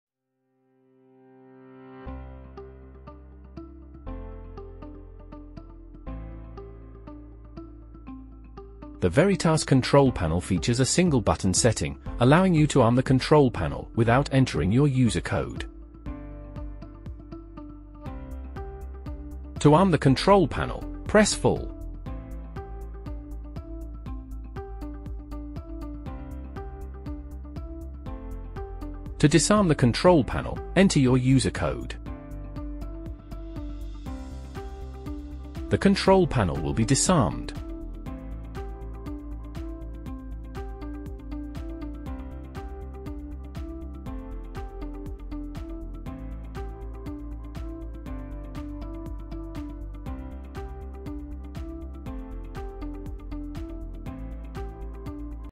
How To Arm & Disarm A Texecom Veritas Security System
Welcome to our tutorial on utilizing the single button setting feature of the Veritas control panel for effortless arming and disarming. With this straightforward guide, you'll master the process in no time.

Arming the Control Panel:
To arm the control panel swiftly, simply press the 'full' button. No need to enter your user code—just one press and you're protected.

Disarming the Control Panel:
When it's time to disarm, enter your user code to restore normalcy. It's that simple.

Stay in Control:
With the Veritas control panel's single button setting, managing your security has never been easier.

Join us in simplifying your security system operations. Watch the video, master the process, and stay in control of your security.

Get Involved:
Stay secure and in the know with our easy-to-follow guides! Liked our video?